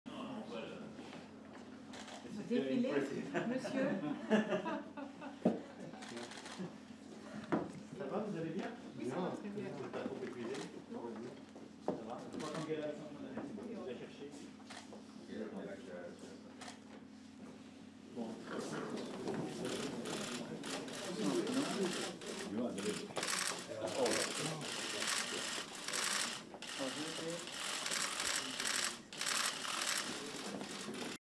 21.02.2020 - Ursula von der Leyen, Emmanuel Macron & Charles Michel - EU-Sonderrat (2. Tag)
Schnittbilder zum Treffen der Präsidentin der Europäischen Kommission, des französischen Staatspräsidenten und des Präsidenten des Europäischen Rates am zweiten Tage des außerordentlichen Europäischen Rates in Brüssel vom 21. Februar 2020